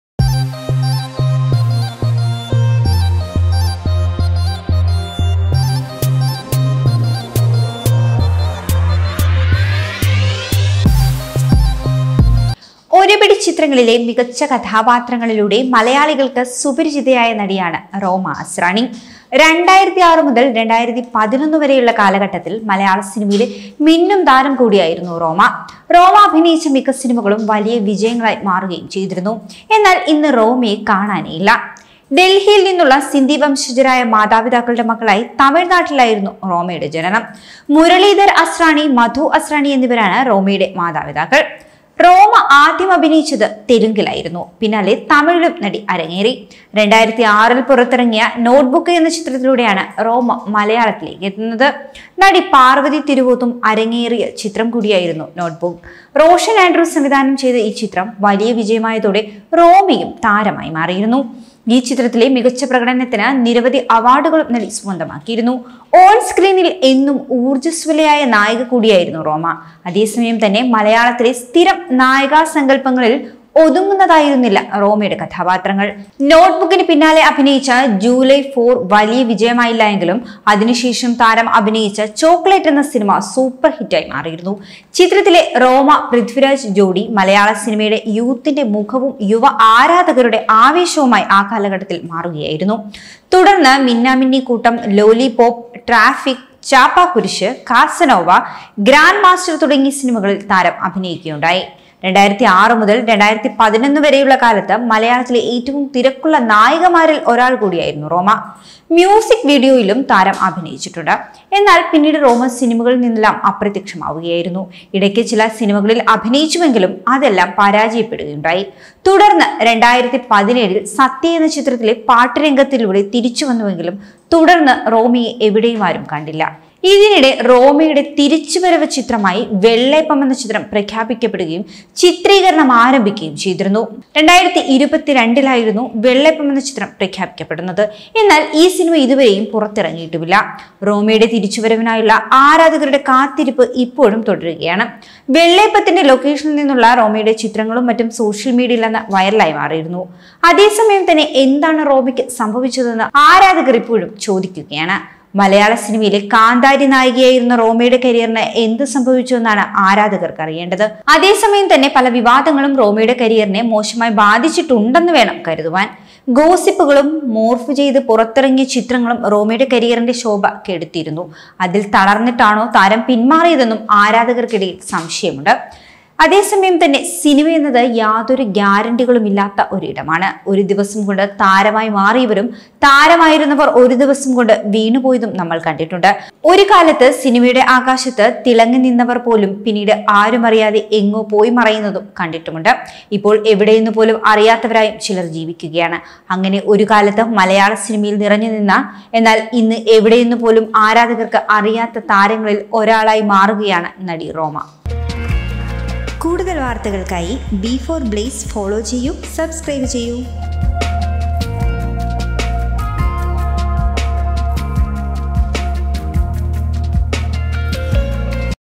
ഗോസിപ്പുകളും മോർഫ് ചെയ്ത ചിത്രങ്ങളും റോമയെ തളർത്തിയോ ? ; നടി എവിടെയെന്ന് ആരാധകർ
ഇതിനിടെ റോമയുടെ തിരിച്ചു വരവ് ചിത്രമായി വെള്ളേപ്പം പ്രഖ്യാപിക്കപ്പെടുകയും ചിത്രീകരണം ആരംഭിക്കുകയും ചെയ്തിരുന്നു. 2022 ലായിരുന്നു വെള്ളേപ്പം പ്രഖ്യാപിക്കപ്പെടുന്നത്. എന്നാല്‍ ഈ സിനിമ ഇതുവരേയും പുറത്തിറങ്ങിയിട്ടില്ല.